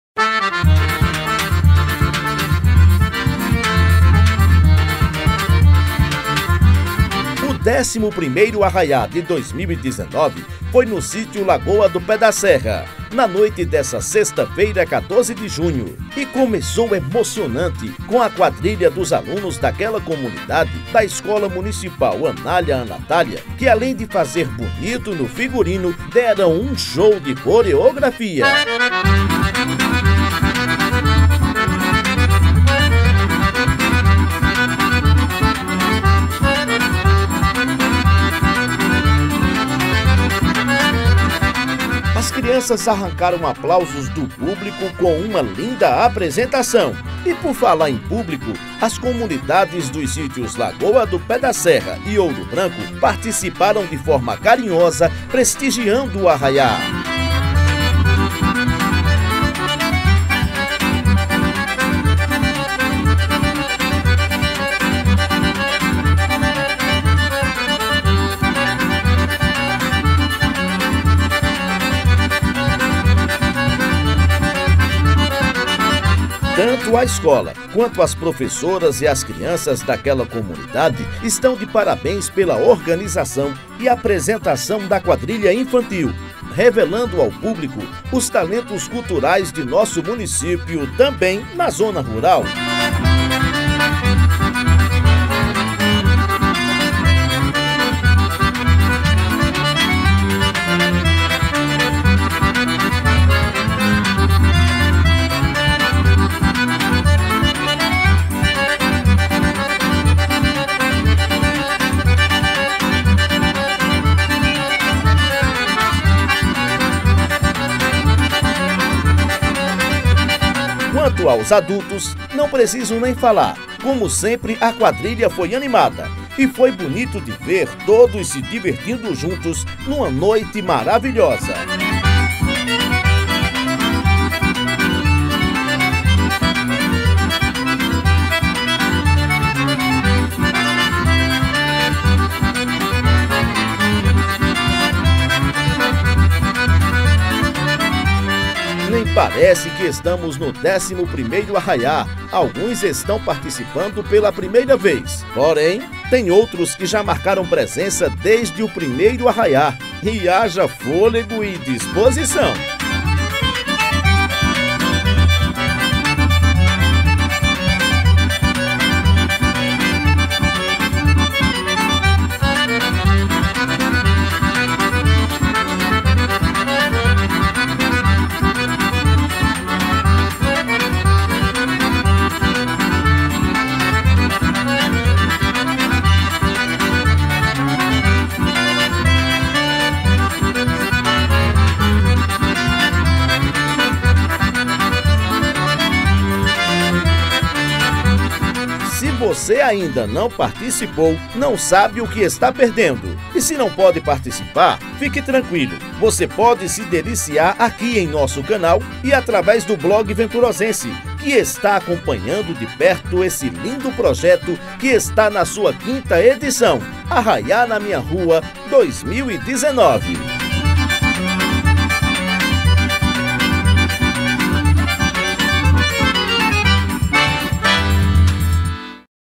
Arraiá na Minha Rua 2019 - Sítio Lagoa do Pé da Serra
O décimo primeiro Arraiá de 2019 foi no sítio Lagoa do Pé da Serra, na noite de sexta-feira, 14 de junho, e começou emocionante com  a quadrilha dos alunos daquela comunidade, da Escola Municipal Anália Anatália de Araújo  que, além de fazer bonito no figurino, deram um show de coreografia. Quanto aos adultos, não preciso nem falar, como sempre a quadrilha foi animada e foi bonito de ver todos se divertindo juntos numa noite maravilhosa, com o projeto Arraiá na Minha Rua 2019!